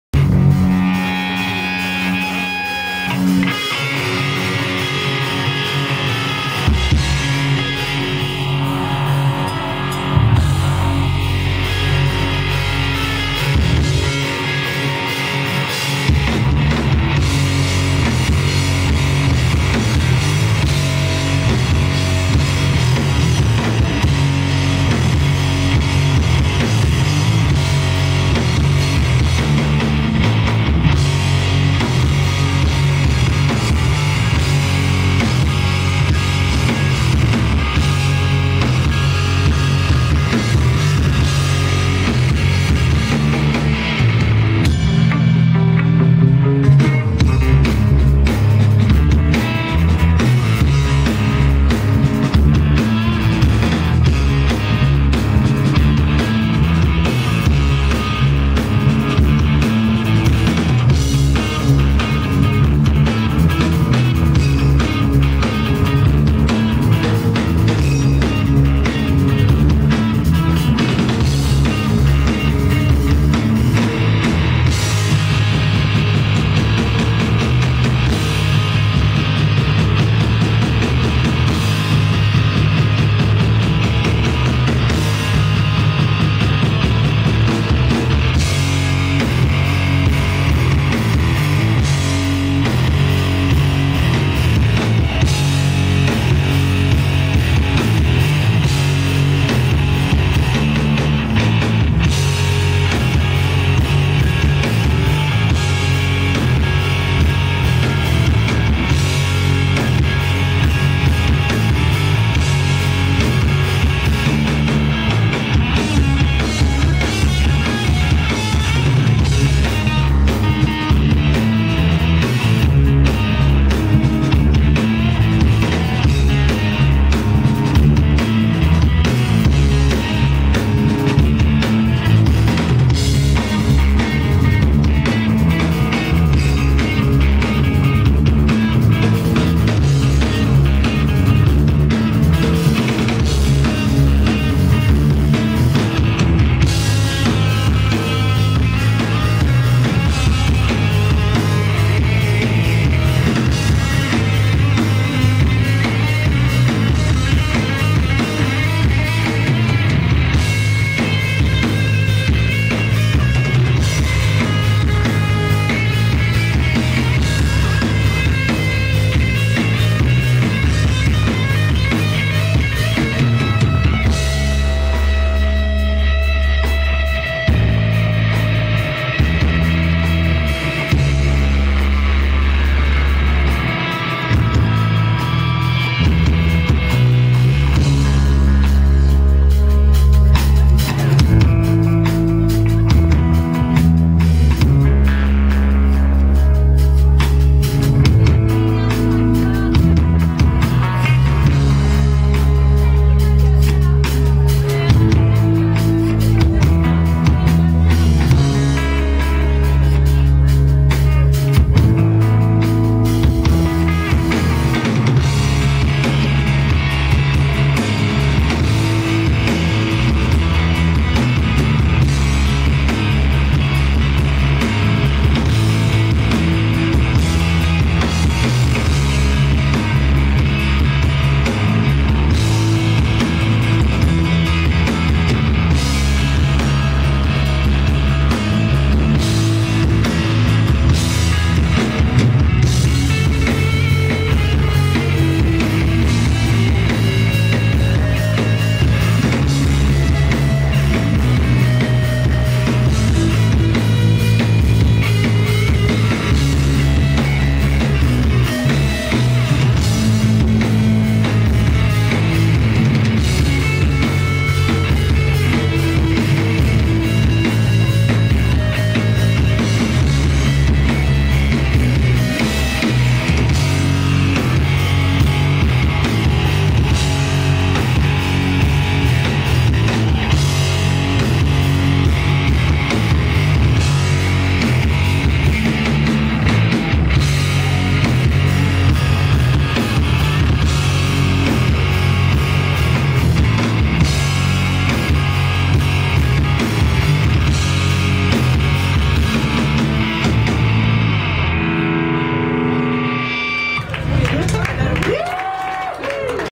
AANDO 2 (Szabad az á, Budapest, HU)
AANDO playing at Szabad az á in Budapest, Hungary on April 1st, 2011.